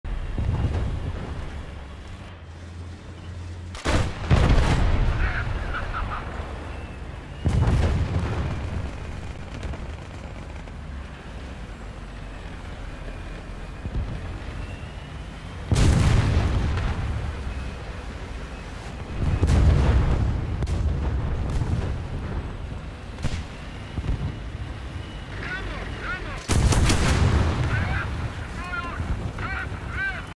These darts... Don't do much
The amount of damage and spalling a 40mm dart does is equal to that of throwing the dart at the enemy